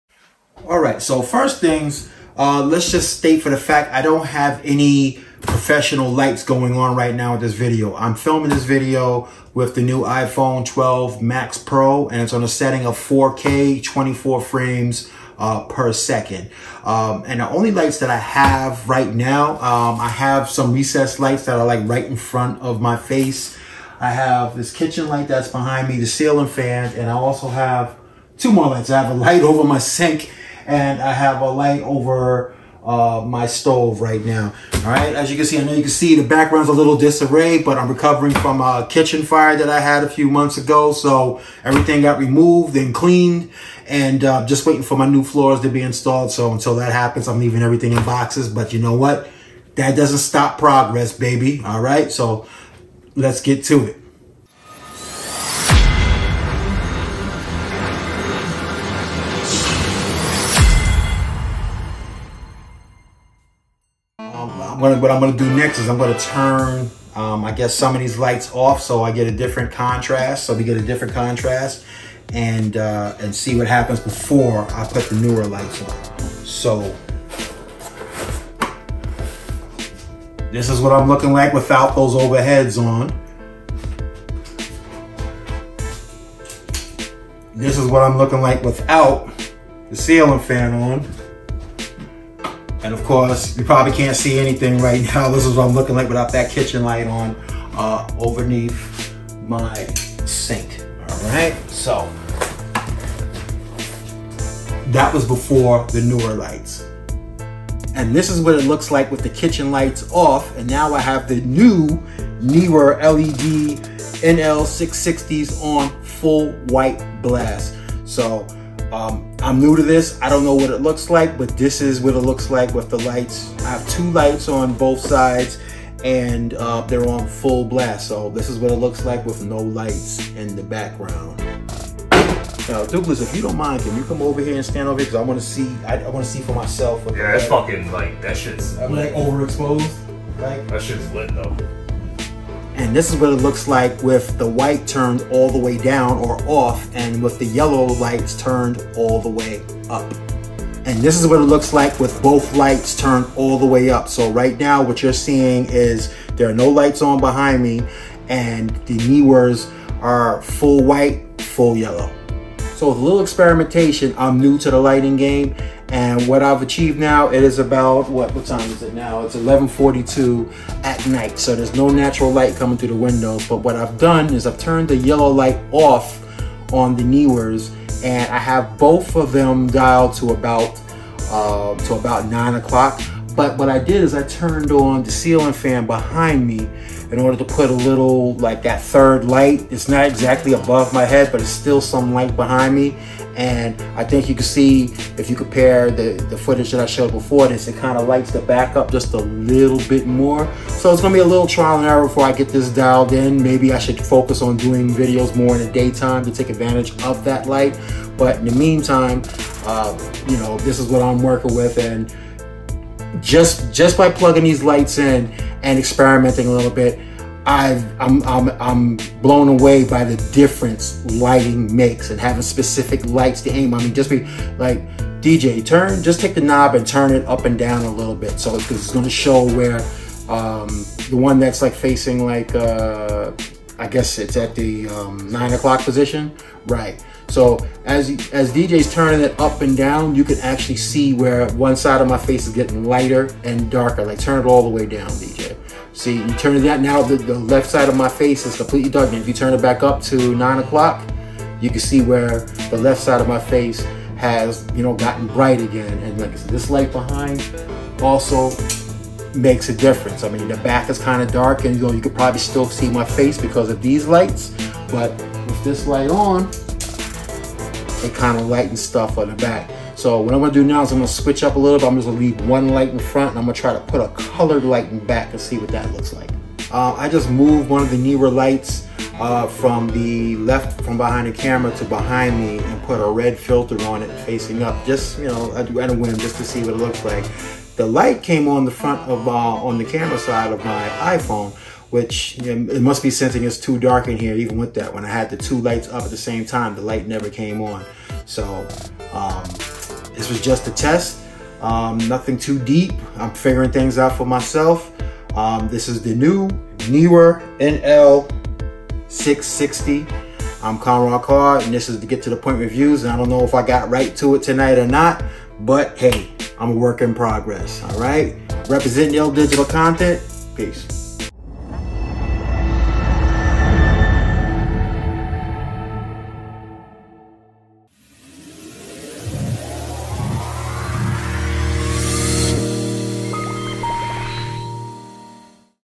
Neewer 660 LED Video and Photography Lights (Before and After Comparison)
This channel focuses on reviewing FOOD, PRODUCTS and TECHNOLOGY that I use in my personal life everyday.

This video will show what the difference is before and after I use NEEWER LED-NL660 Photography Lights in my kitchen. The before and after comparison is AMAZING!!!